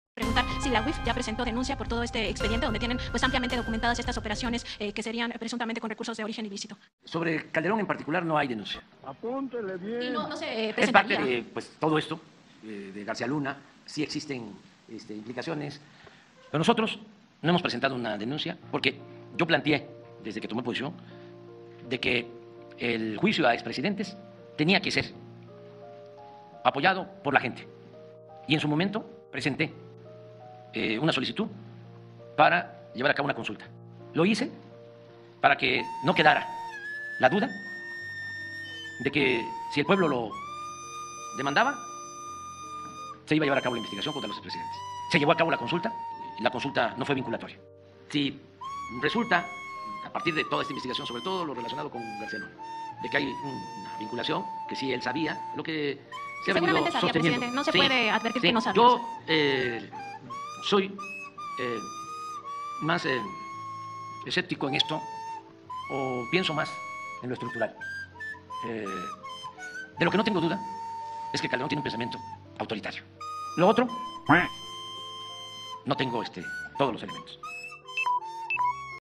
No hay denuncia contra Calderón y el GobiernoMx no presentará 🦆
No hay denuncia contra Calderón y el GobiernoMx no presentará, porque la consulta para enjuiciar a expresidentes no fue vinculatoria 🦆